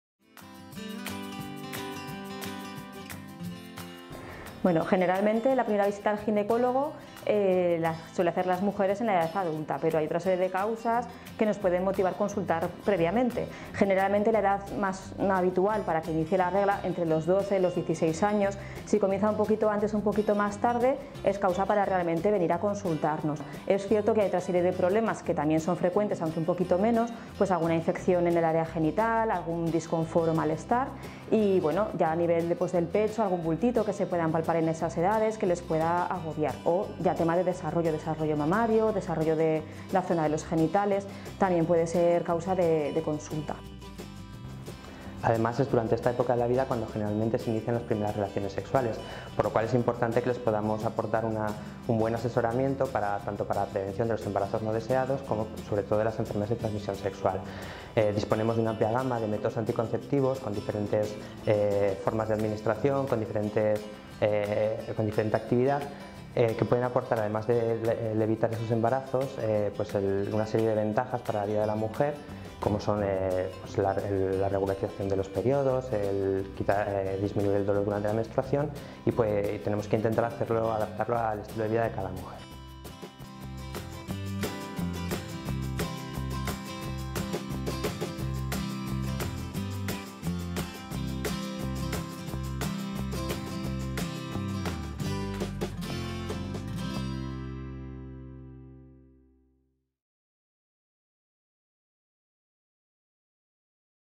Ginecología en la adolescencia
La pubertad, en la adolescencia, marca la puesta en funcionamiento de las hormonas sexuales. Comienza una época de cambios ante los cuales es recomendable una primera visita al ginecólogo. Además, en este momento es importante conocer información tanto de la prevención de enfermedades de transmisión sexual, así como de métodos anticonceptivos.